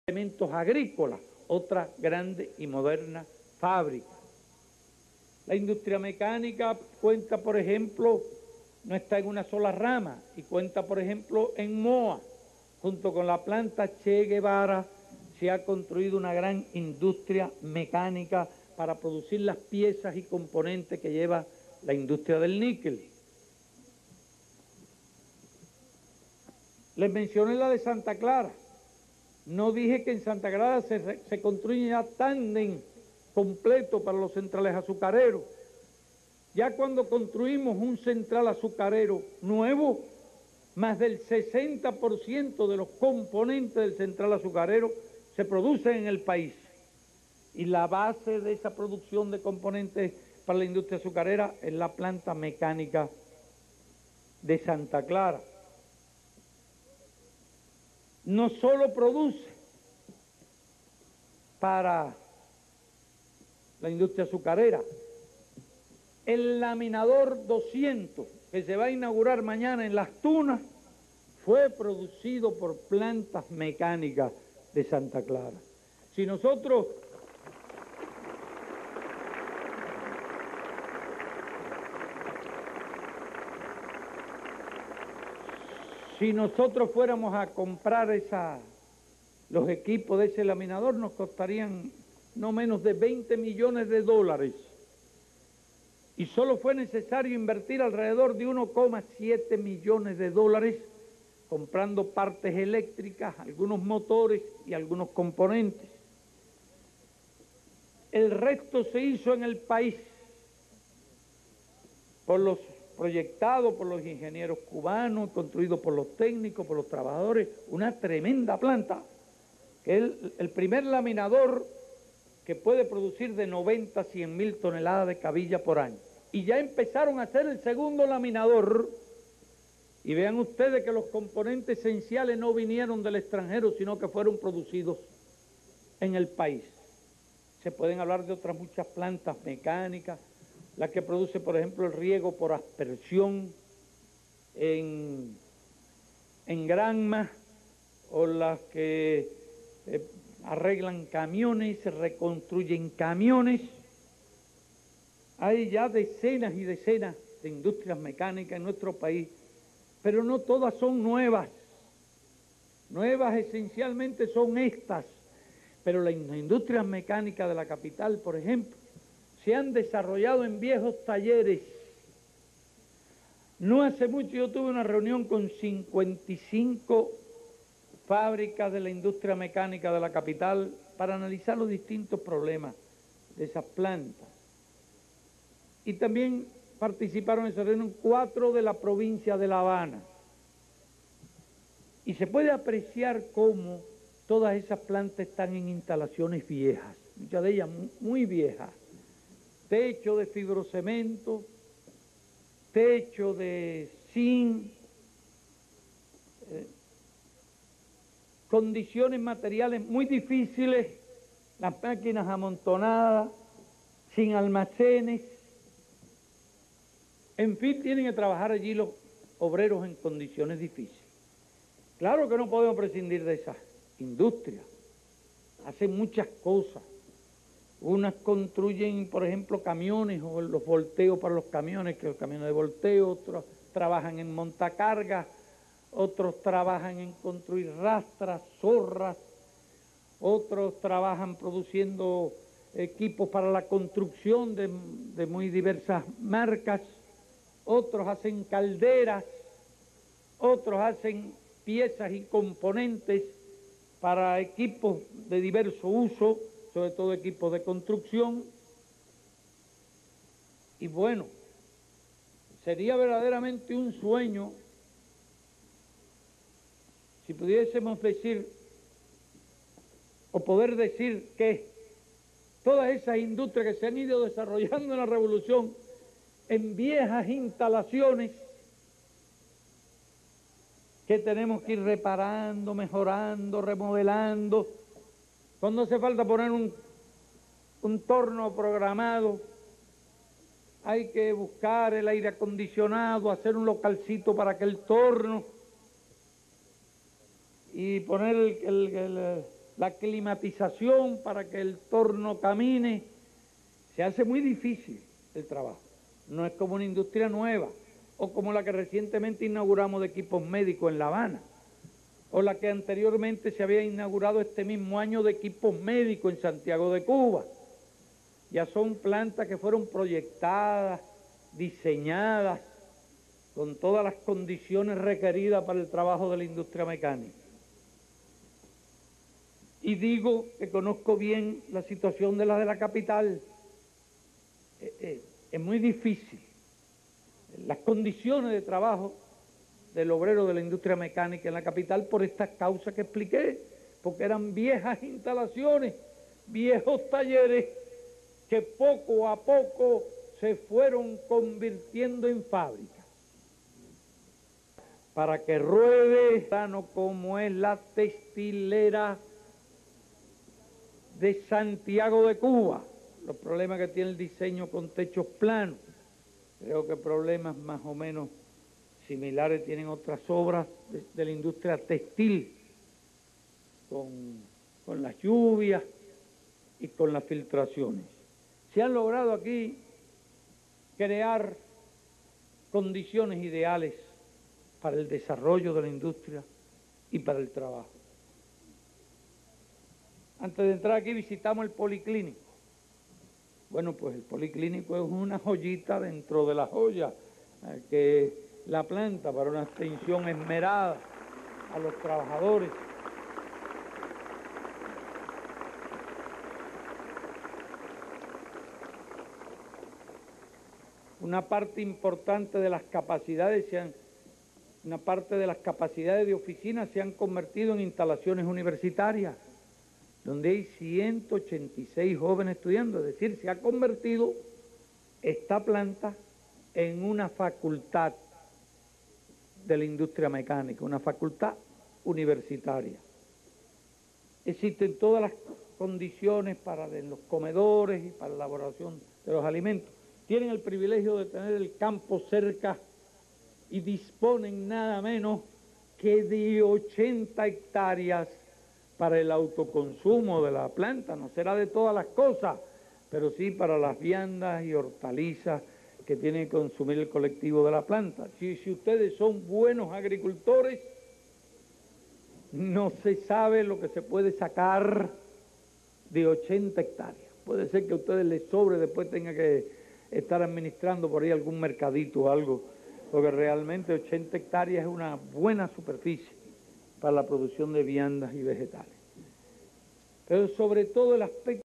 Discurso de Fidel Castro en Planta Mecánica
El Comandante en Jefe inauguró el 27 de noviembre de 1988 este enclave que se ha convertido en una importante industria para la producción nacional de bienes y servicios.